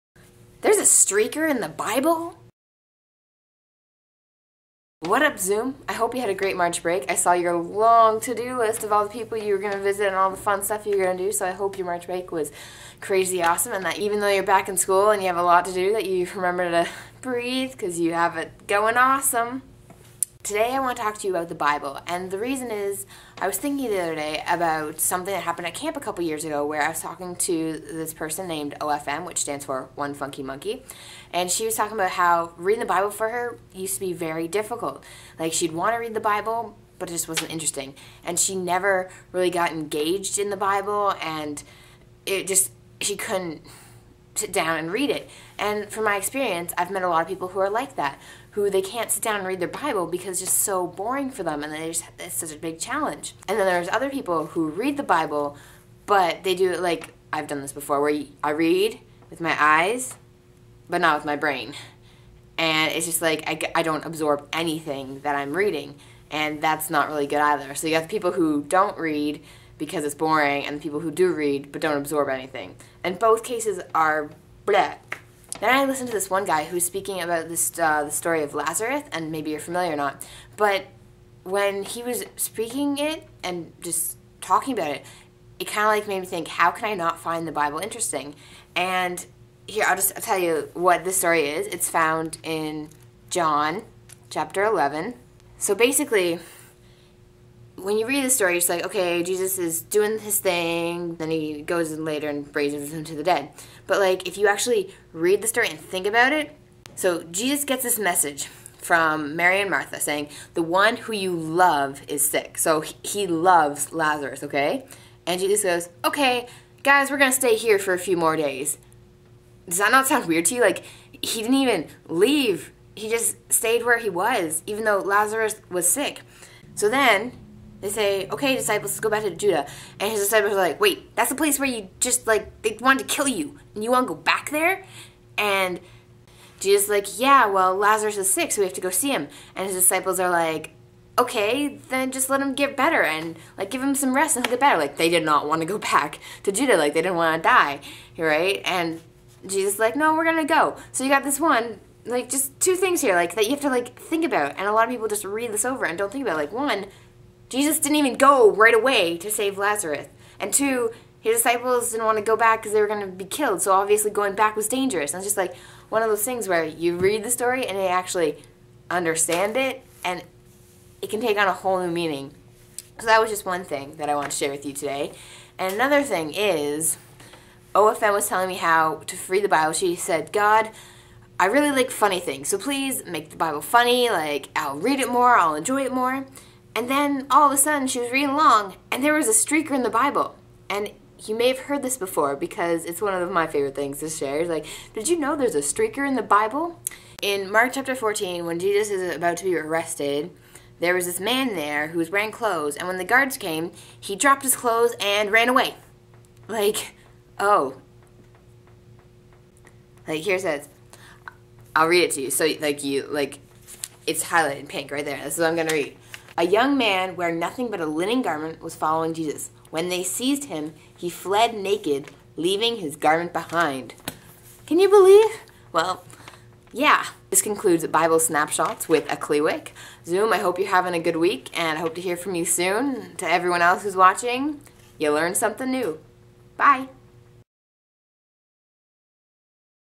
Bible Snapshots - There's a Streaker in the Bible!
Just talking to Zoom about the Bible and why some people don't read it and why some people have trouble reading it...

Oh yeah, and about the Streaker in the Bible! =P